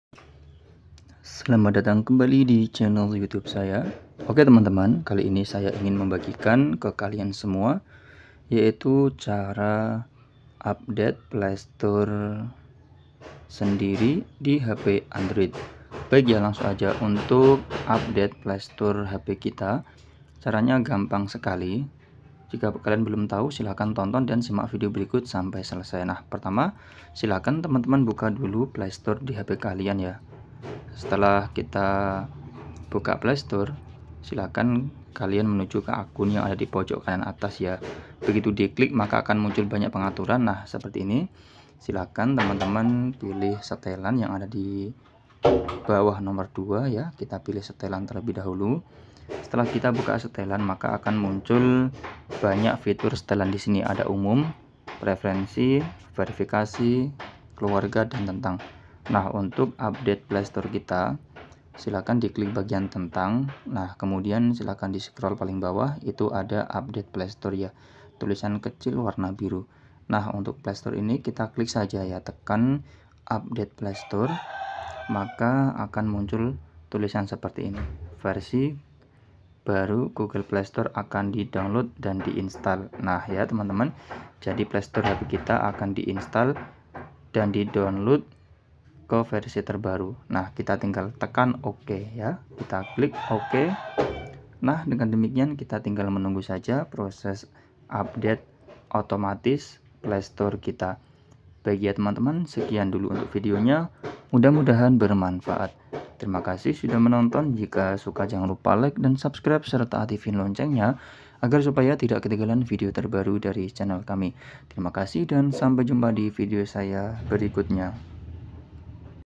cara update playstore versi terbaru di semua android
berikut ini saya bagikan cara update playstore versi terbaru. oke langsung saja untuk video nya sudah kami jelaskan di video ini silahkan tonton dan simak videonya sampai selesai semoga bermanfaat , terimakasih.